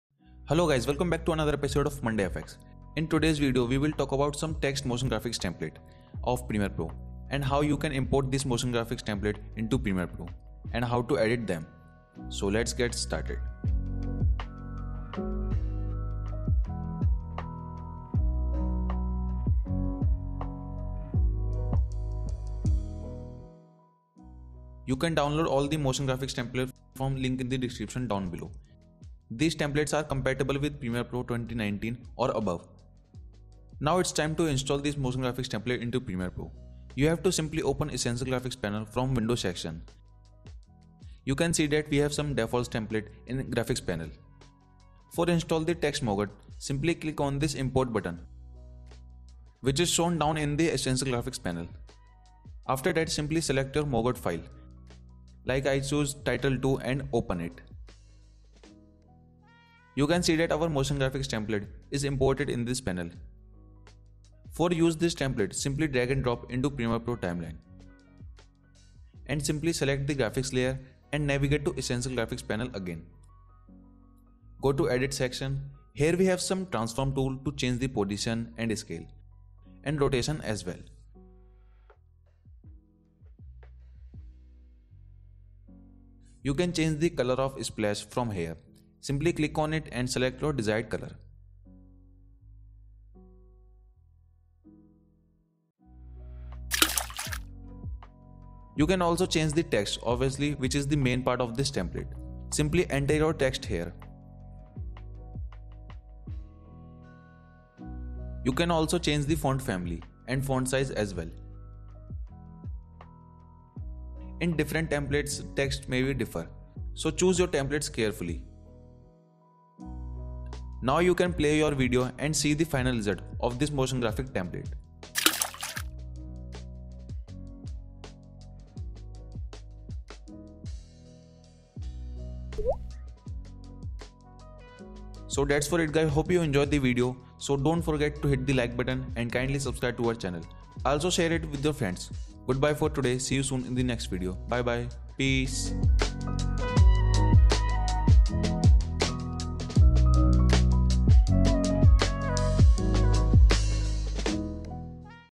Free Text MOGRT for Premiere Pro CC 2019+
Hello guys
Today you will learn "How to use MOGRT file in Premiere Pro CC 2020"...

Instructor:- Jitesh Verma
Editor:- Jitesh Verma